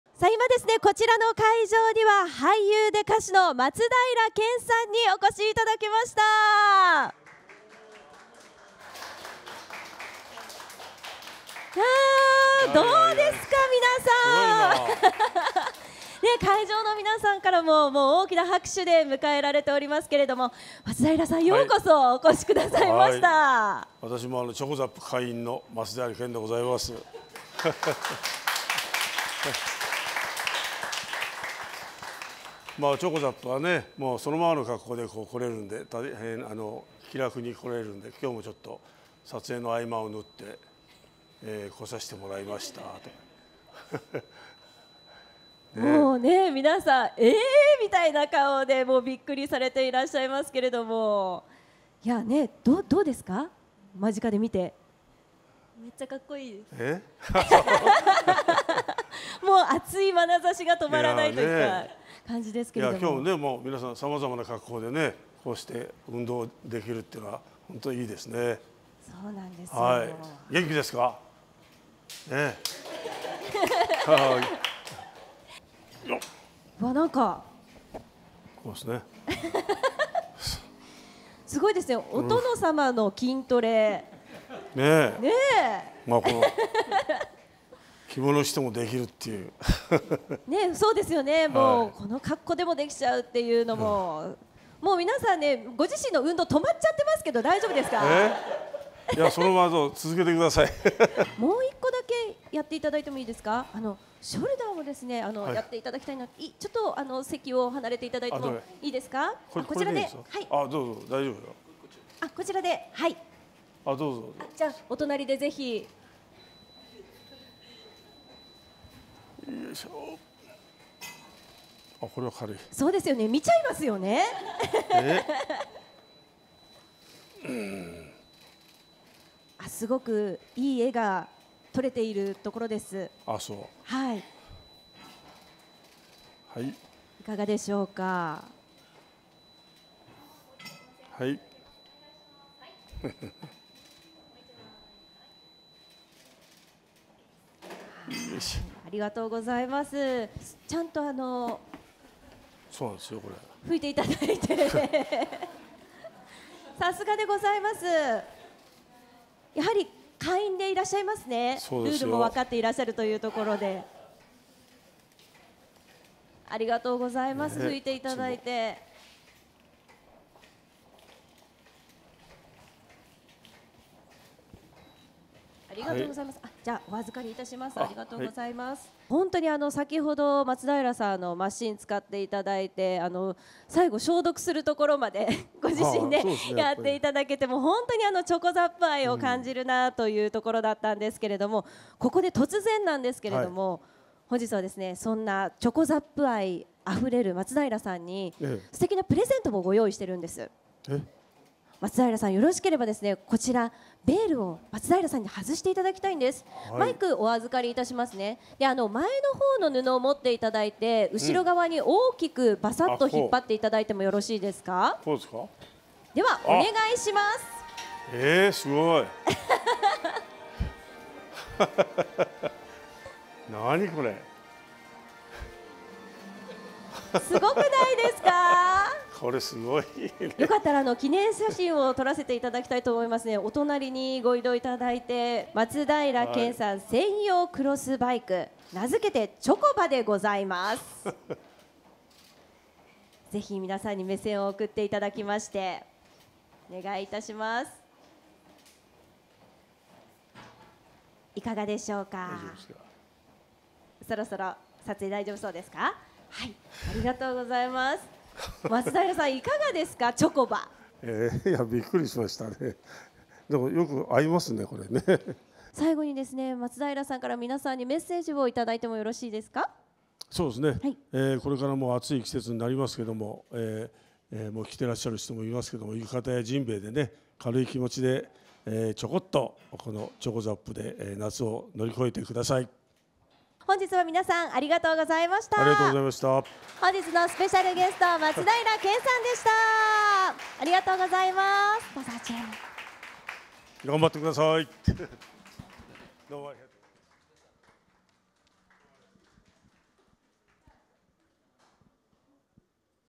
松平健、サプライズ登場で驚き！？女性からの「かっこいい」に照れる場面も RIZAPのコンビニジム『chocoZAP』PRイベント
俳優で歌手の松平健が12日、都内で開催されたRIZAPのコンビニジム『chocoZAP』のPRイベントに出席。『chocoZAP』店舗をサプライズ訪問し、会場を驚かせた。